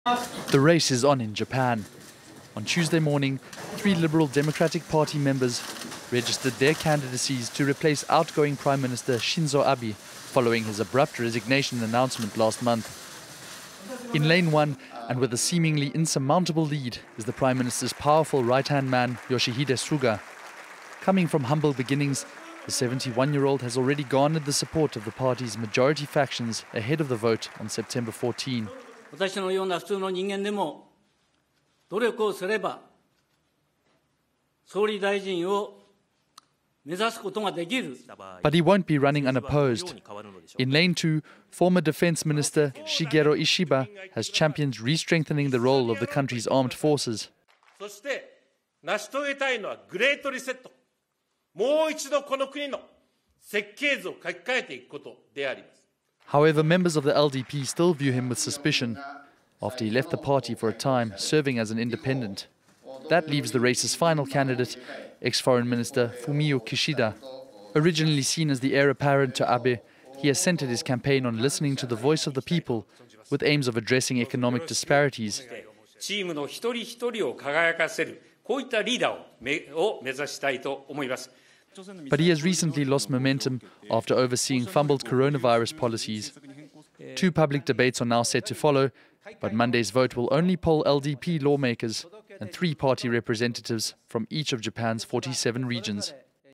Japan leadership contest: Campaigning begins in race to succeed PM Shinzo Abe
Campaign to succeed PM Abe as party leader begins in Japan.